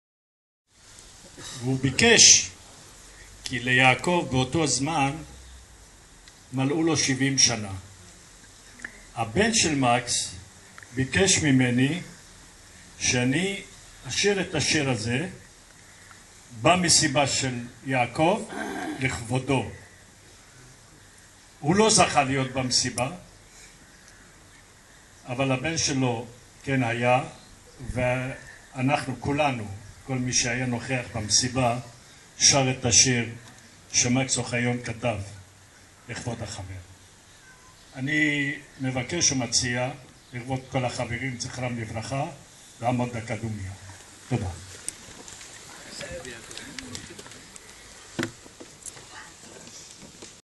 פגישה שנתית של הגרין (שבת ראשון אחרי יום עצמאות)
דקה דומיה לזכר החברים .ז"ל